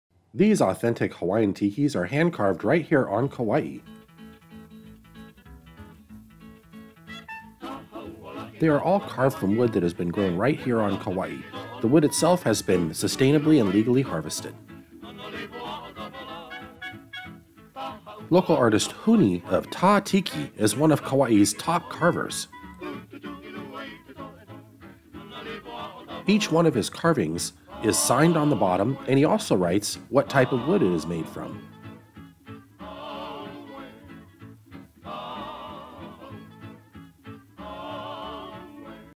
Ta Tiki at The Kauai Store
These authentic tiki's are made on Kauai by master carver Huni.  Each piece is signed on the bottom by the artist who also notes what type of wood it is.